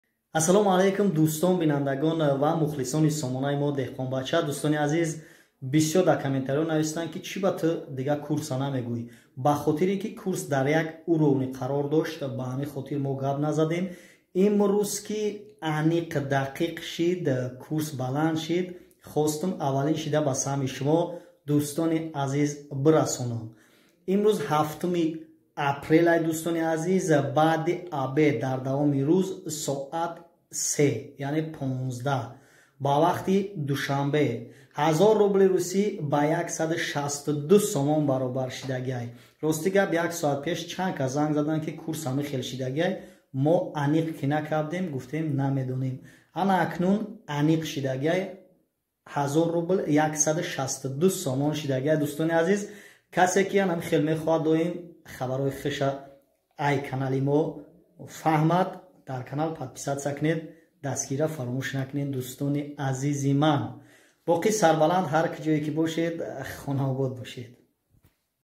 Курби асъор рублей ба 162 сомони расид имруз 7 апреля 2022 г. Хушхабар
Подпишитись на наш канал и будьте в курсе наших новых видео